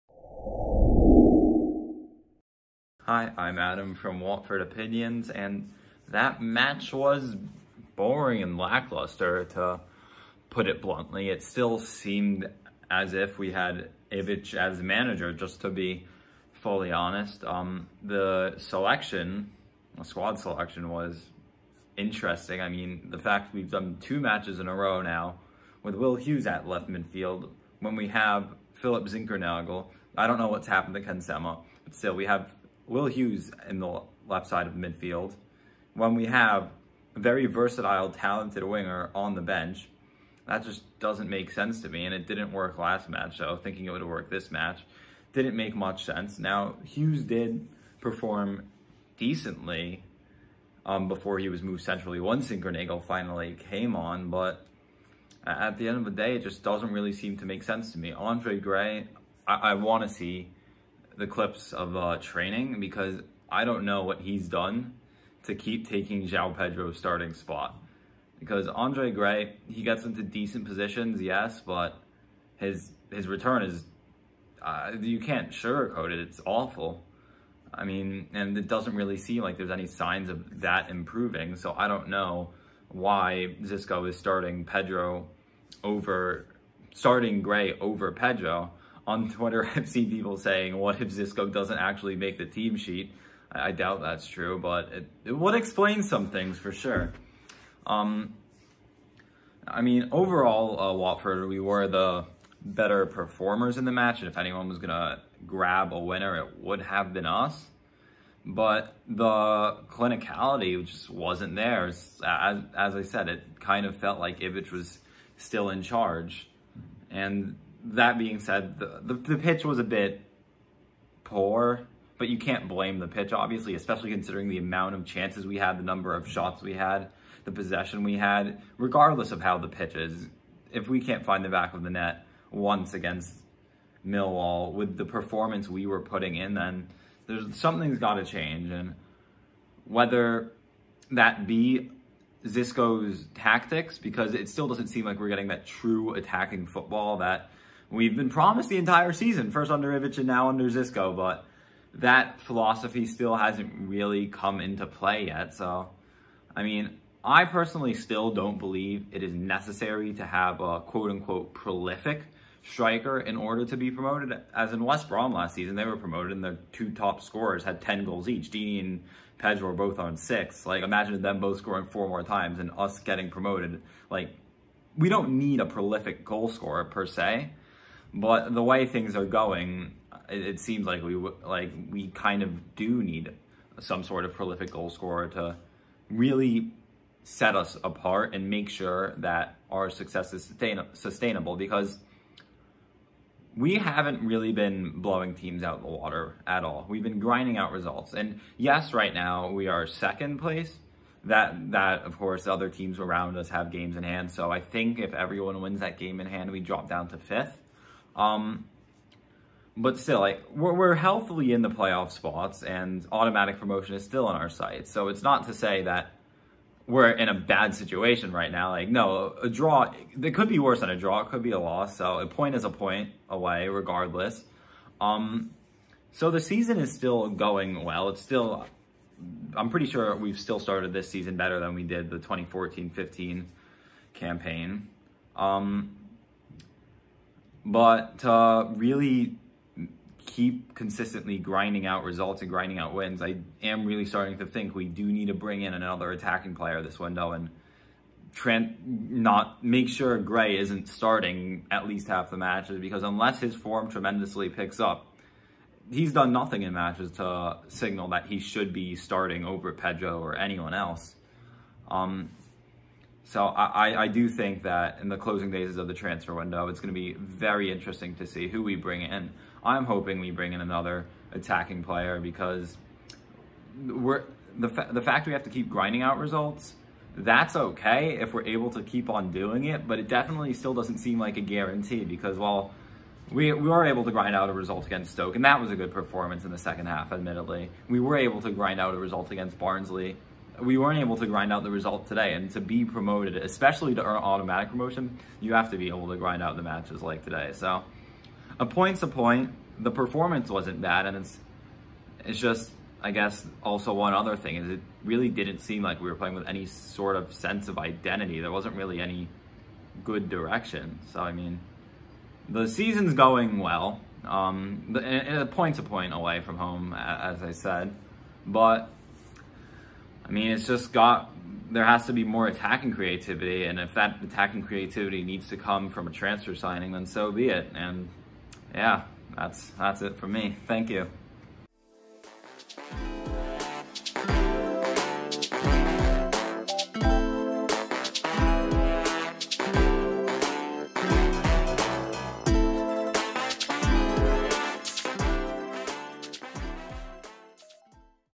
Millwall 0-0 Watford | Match Reaction
Watford moved up to second in the Sky Bet Championship following a goalless draw against Millwall at The Den.

Adam Katcher gives his immediate thoughts after the match and how Watford need to improve going forward.